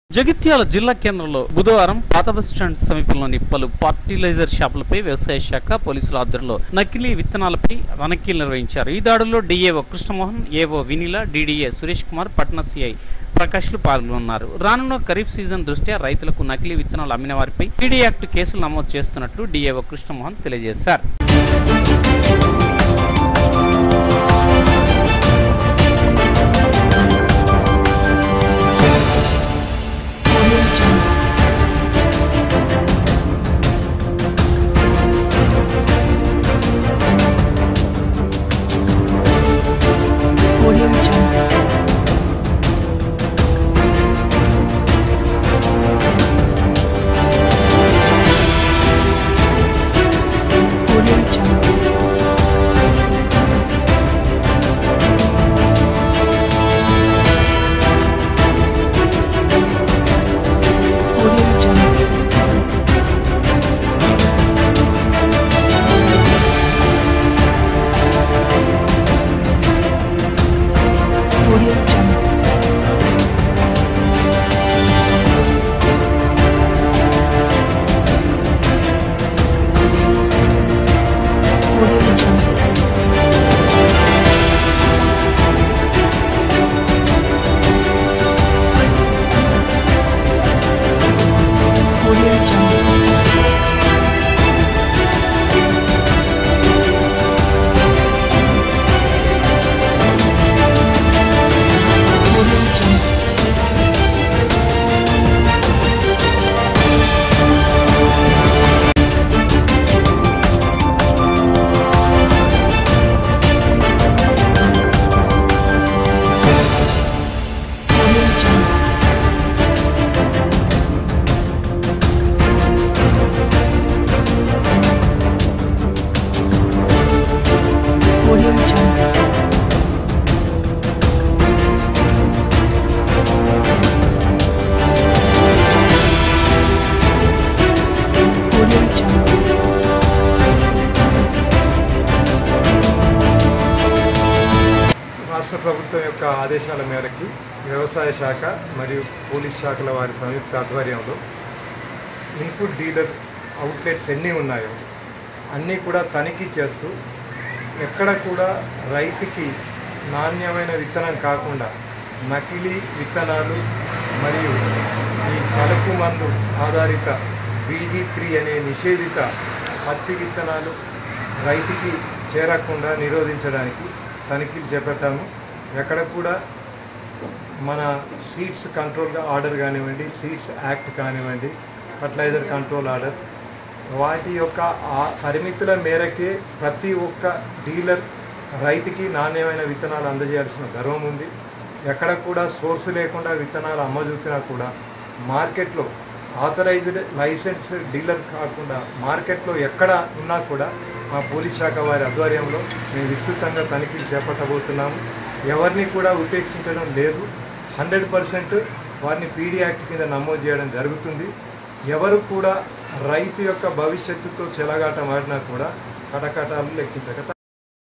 22.05.19| DSC | Jagtial |జిల్లా కేంద్రంలో వ్యవసాయ శాఖ ఆధ్వర్యంలో ఫర్టిలైజర్ షాపులపై దాడులు
| Durga Siti Cable | News |జిల్లా కేంద్రంలో వ్యవసాయ శాఖ ఆధ్వర్యంలో ఫర్టిలైజర్ షాపులపై దాడులు